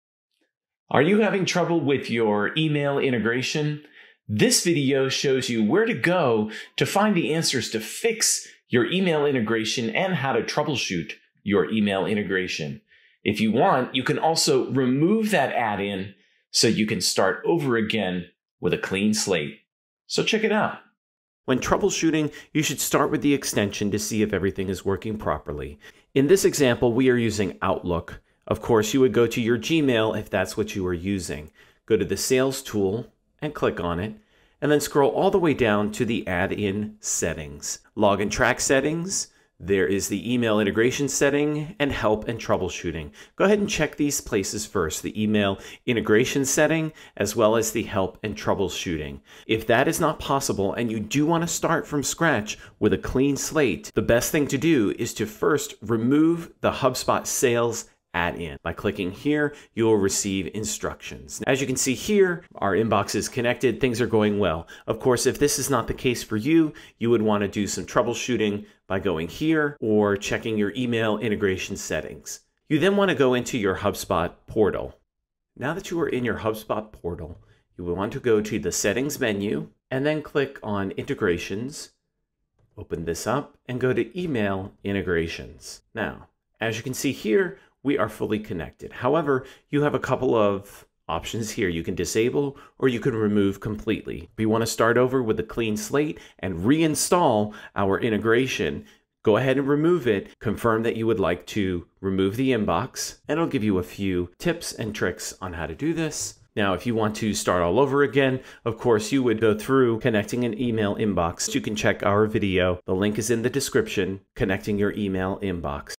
How To Troubleshoot Your HubSpot Email Integration
Having trouble with your HubSpot email integration? This video shows you how to troubleshoot and (if necessary) remove the integration so you can start again.
You may be interested in these videos as well:

👩‍💻Let us know what you would like to see in future videos and tutorials.

✍️ TRANSCRIPT:
Are you having trouble with your email integration? This video shows you where to go to find the answers to fix your email integration and how to troubleshoot your email integration. If you want, you can also remove that add-in so you can start over again with a clean slate. So check it out. When troubleshooting, you should start with the extension to see if everything is working properly. In this example, we are using Outlook. Of course, you would go to your Gmail if that's what you were using. Go to the sales tool and click on it, and then scroll all the way down to the add-in settings. Log and track settings, there is the email integration setting, and help and troubleshooting. Go ahead and check these places first, the email integration setting as well as the help and troubleshooting. If that is not possible and you do wanna start from scratch with a clean slate, the best thing to do is to first remove the HubSpot Sales add-in. By clicking here, you will receive instructions. As you can see here, our inbox is connected, things are going well. Of course, if this is not the case for you, you would wanna do some troubleshooting by going here or checking your email integration settings. You then wanna go into your HubSpot portal. Now that you are in your HubSpot portal, you will want to go to the settings menu and then click on integrations. Open this up and go to email integrations. Now, as you can see here, we are fully connected, however, you have a couple of options, here. You can disable or you could remove completely. We wanna start over with a clean slate and reinstall our integration. Go ahead and remove it, confirm that you would like to remove the inbox. It'll give you a few tips and tricks on how to do this. Now, if you want to start all over again, of course, you would go through connecting an email inbox. Can check our video, the link is in the description, connecting your email inbox.